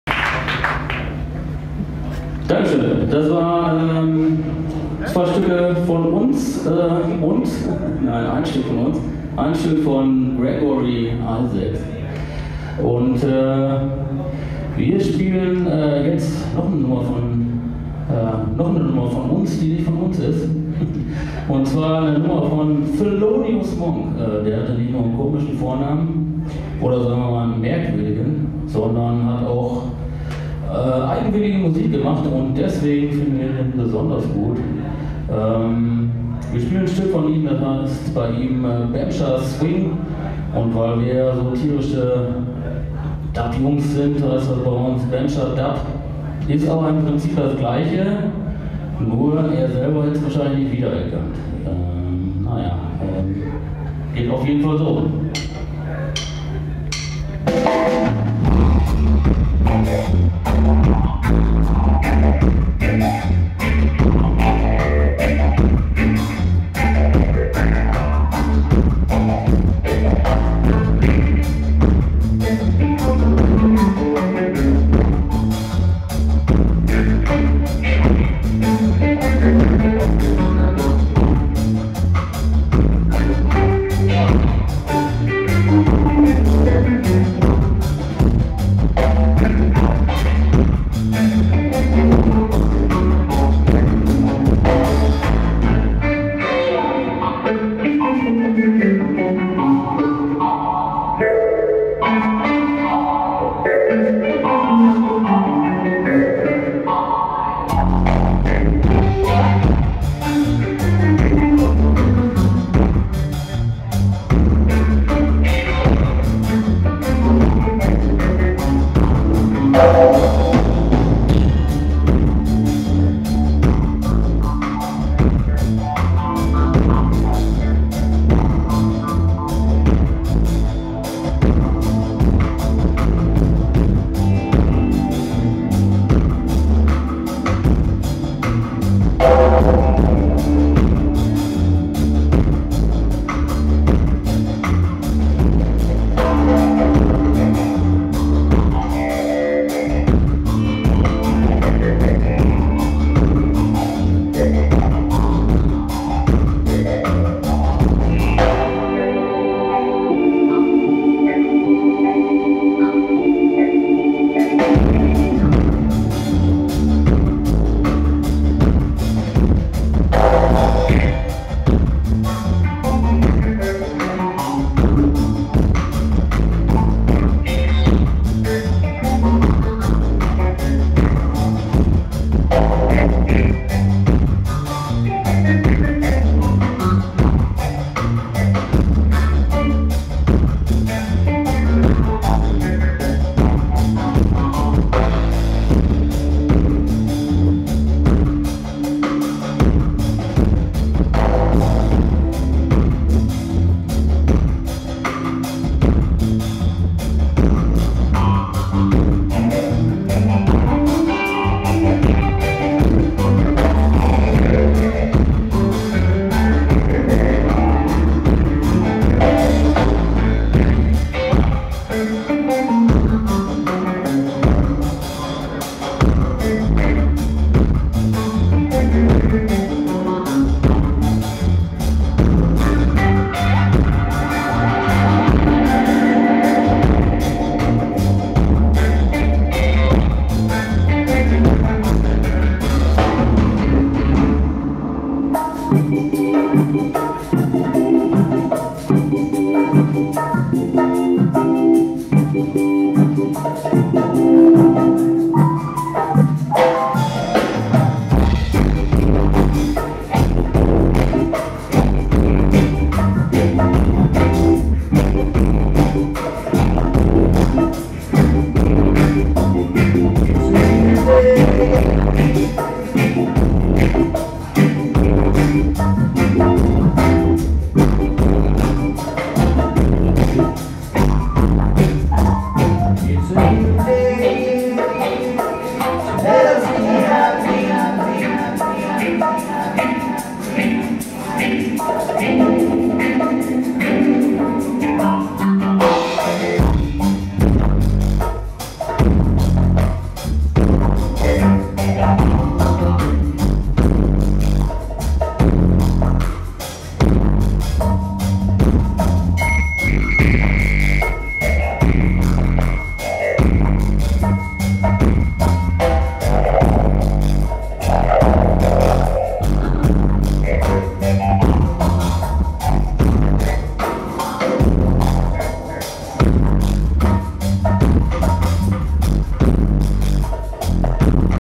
The Senior Allstar in Dub 2. Freedom Sounds Festival, Köln 02 Mai 2014
The Senior Allstars "In Dub" 2. Freedom Sounds Festival, Gebäude 9, Köln 02. Mai 2014